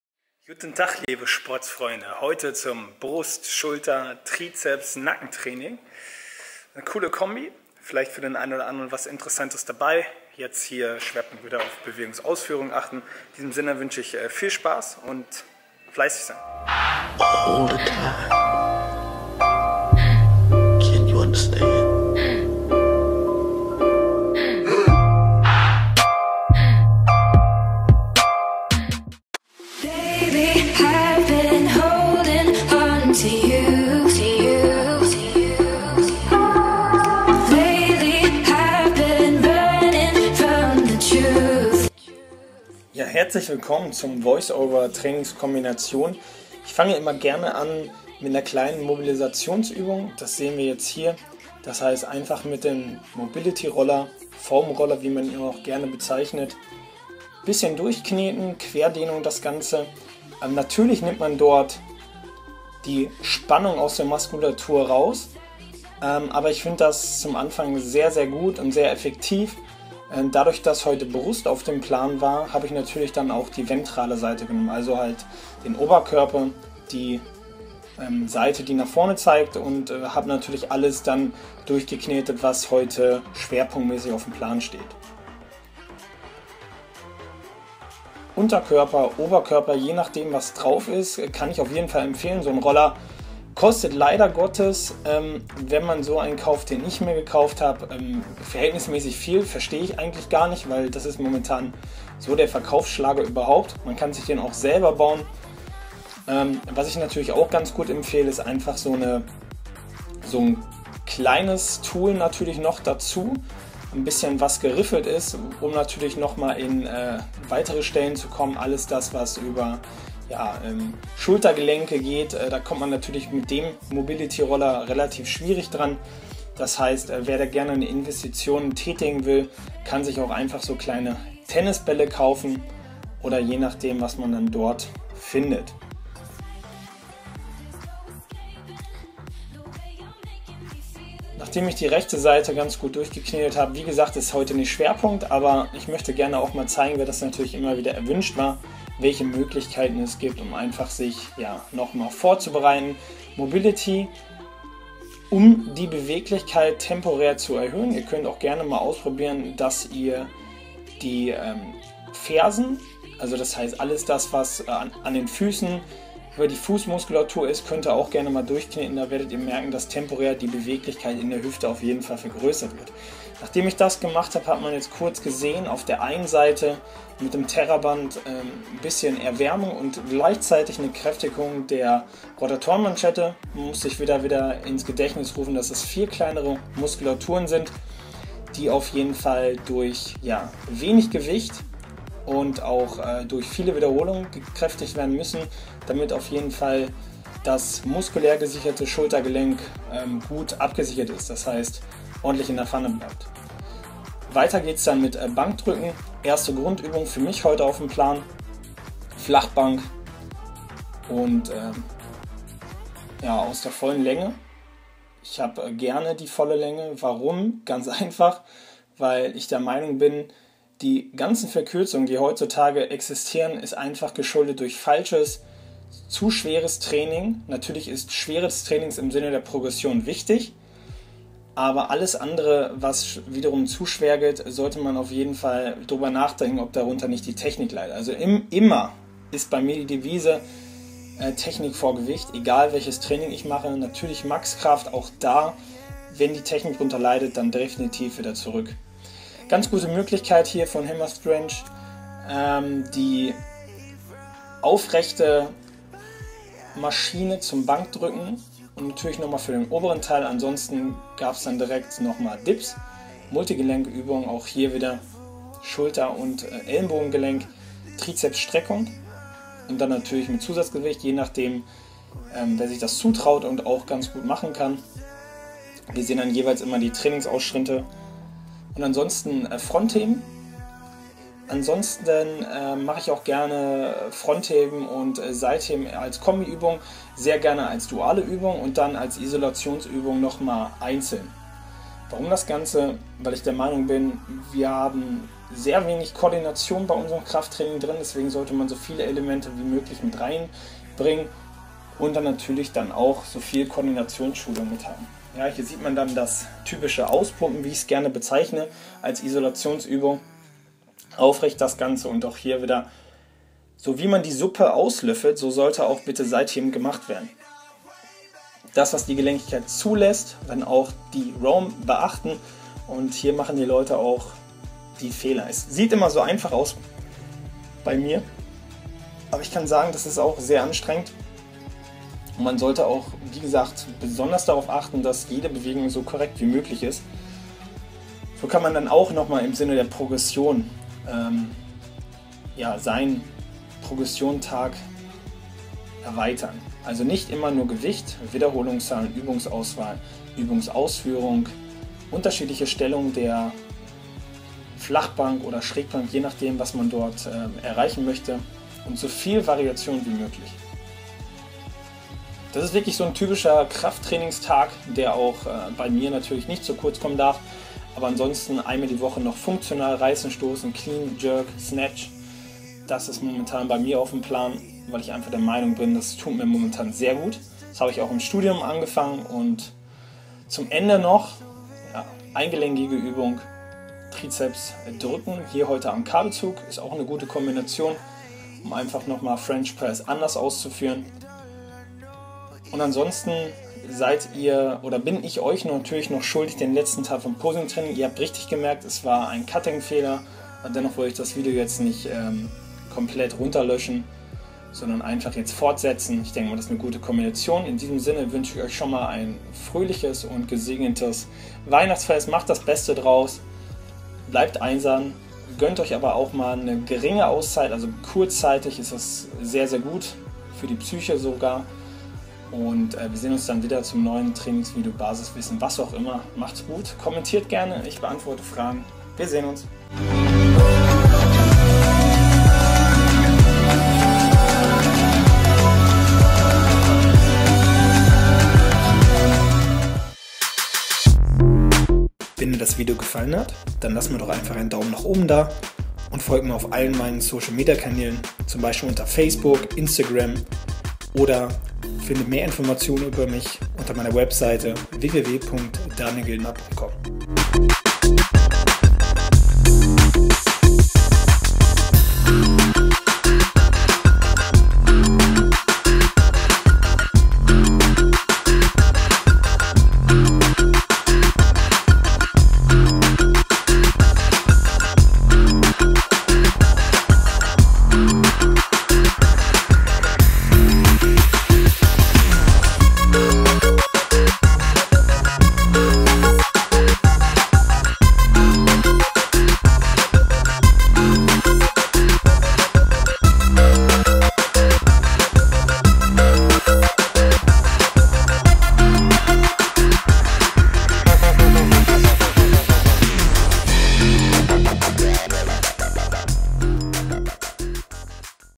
Trainingskombi Brust/Trizeps/Schultern - Warmup + Mobility Full Training VLOG
Heute stand wieder eine Trainingskombi auf dem Plan. Zusätzlich noch ein kurzer Auschnitt zur Mobility und dem fehlenden Posingupdate von letzte Mal. Euch ein gesegnetes und ruhiges Weihnachtsfest!

Powered by BARBOZA™

► SNAPCHAT! ► gildnerofficial
► KIK! ► dgildnerofficial